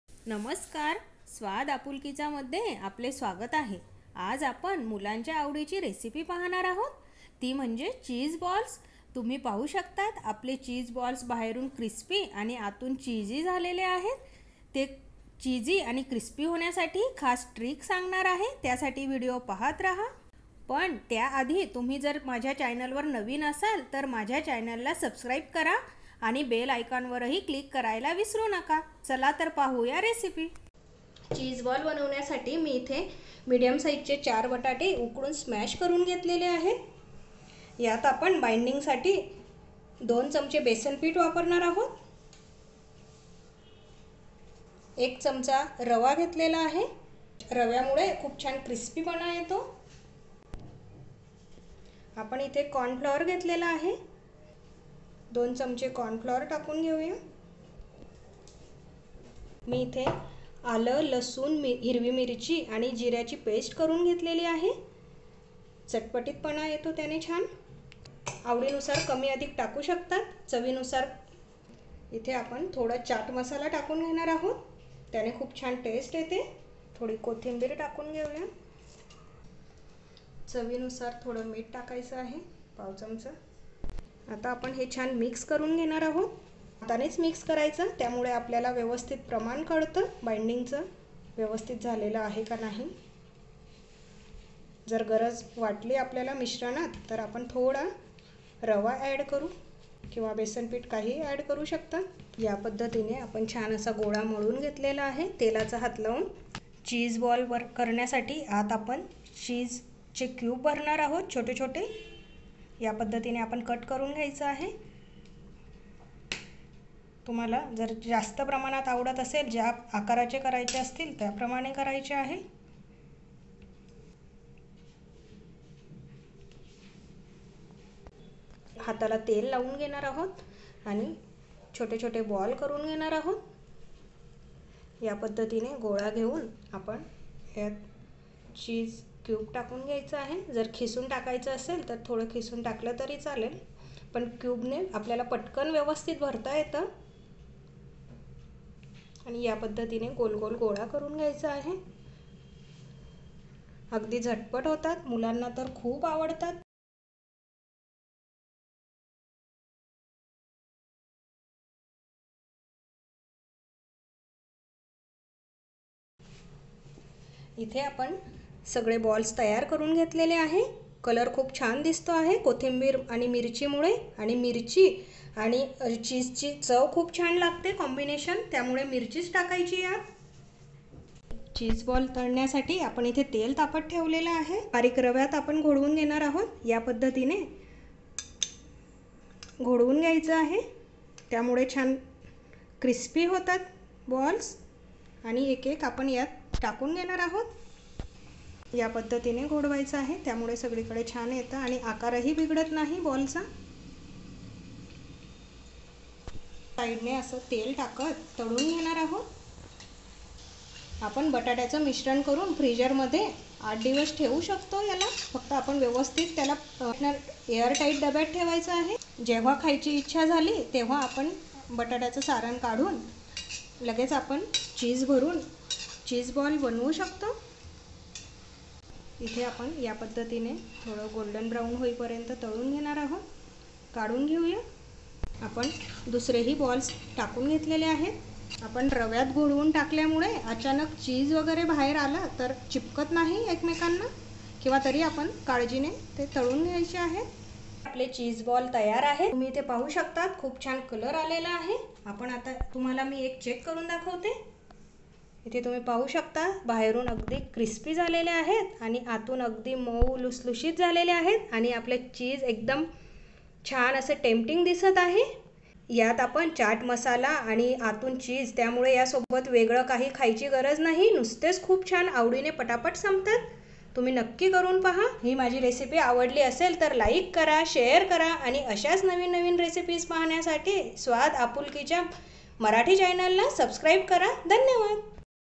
Cheese Balls | Crispy and Cheesy Potato bites | Cheese nugets | चीज बॉल्स | Swaad Aapulkicha
Crispy Cheese Balls | Crispy and Cheesy Potato Snack | चीज बॉल्स | Swaad Aapulkicha |

Today we are preparing kids special recipe that is crispy and cheesy potato balls.

Ingredients :

4 - boiled Potatoes
3 tbsp - gram flour (Besan)
2 tbsp - Semolina (Rava)
1 tsp - Chat masala
Ginger garlic green chilli paste as per taste
Salt as per taste
Cheese